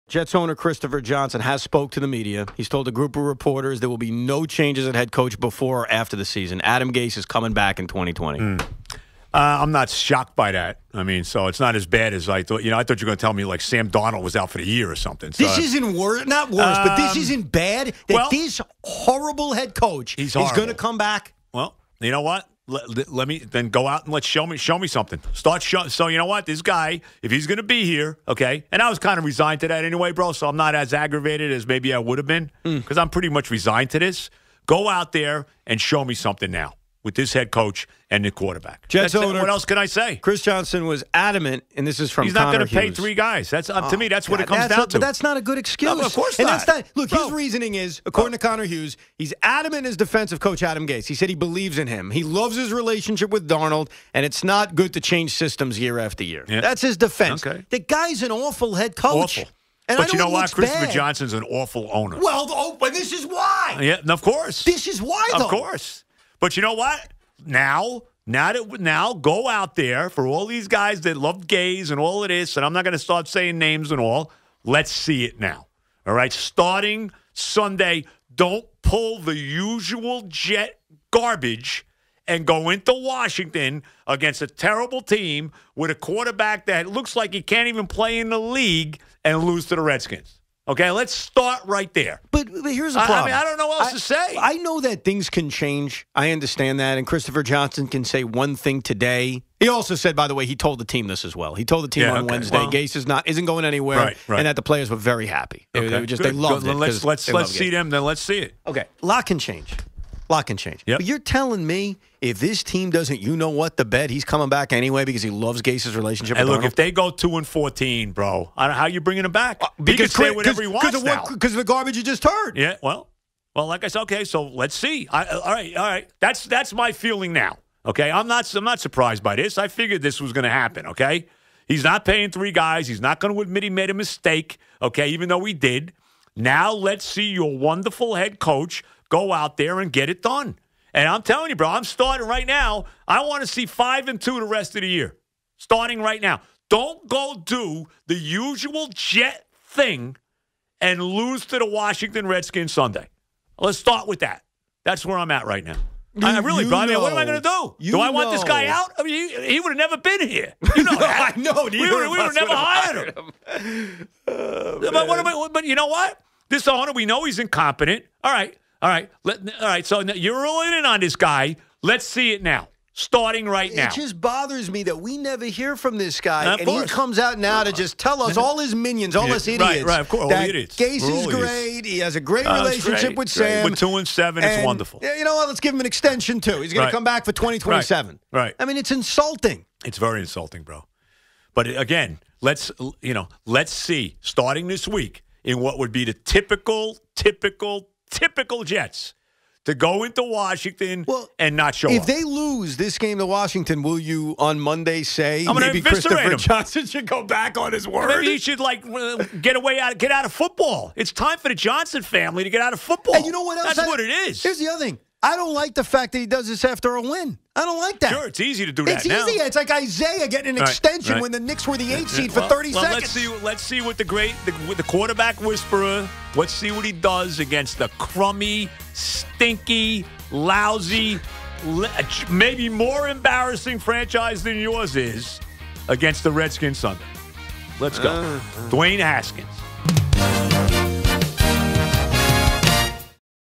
Adam Gase is here to stay! Joe & Evan
Adam Gase's job is safe this season as well as this offseason, Jets CEO Christopher Johnson said Wednesday.

Talking to reporters in Florham Park, New Jersey, Johnson said he found it "exceedingly frustrating" that the Jets, 2-7 this season, have not lived up to his expectations but gave Gase a vote of confidence anyway.

"There’s a great deal of work to do," Johnson said. "Everyone understands that, and we're just getting started. But I feel really good about this team moving forward."

Johnson said he told the team last week that Gase, who was hired in January, would return in 2020

"Adam has the trust of this team. He has the trust of Sam. He has Joe's trust," Johnson said, referring to general manager Joe Douglas. "He has my trust. He's a good man. He's a good coach."

After three straight seasons of double-digit losses, the Jets entered the season with higher expecations due to promising second-year quarterback Sam Darnold and key offseason acquisitions such as running back Le'Veon Bell and linebacker C.J. Mosley. But Darnold missed time with mono early in the season, and after he returned, he struggled badly in losses to the Patriots and Jaguars. Running behind a subpar offensive line, Bell hasn't looked like the All-Pro he was in Pittsburgh. And a groin injury has sidelined Mosley for all but two games.

"We’ve had a lot of adversity," Johnson said. "Sam hasn’t been given the best opportunity to thrive. We’ve had so many moving parts on the offensive line that I think it has affected his ability to play as he would like."

The Jets next play Sunday at 1-8 Washington.